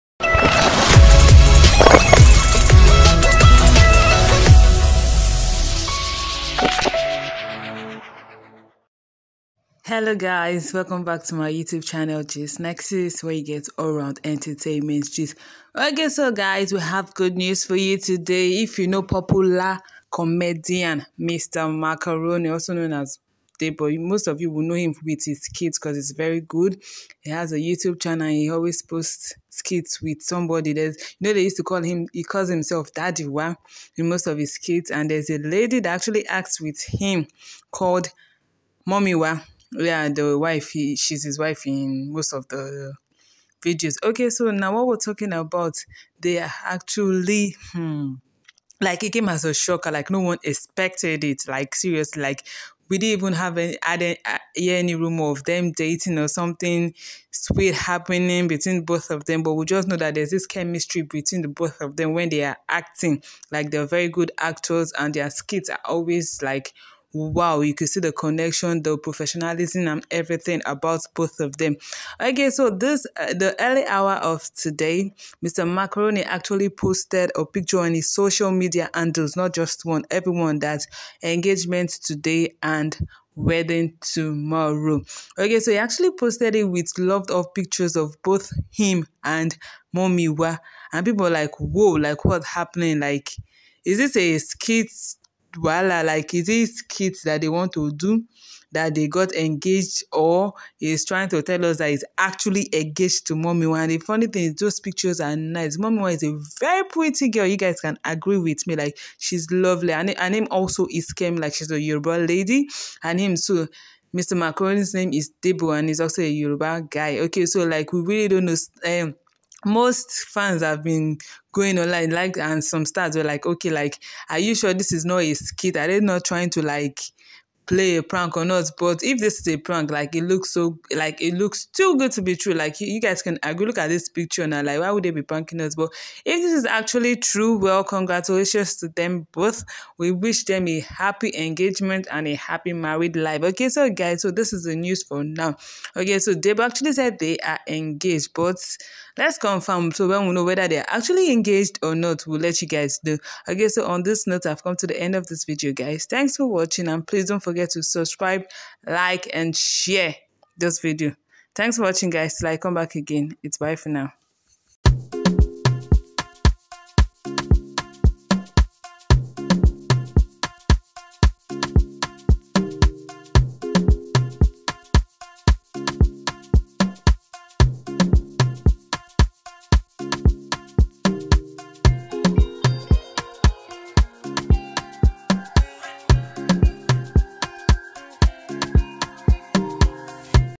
Mr Macaroni Secret Engagement / Wedding Video With Mummy Wa ( Full Video )
Mr Macaroni gets married to mummy was
Full Video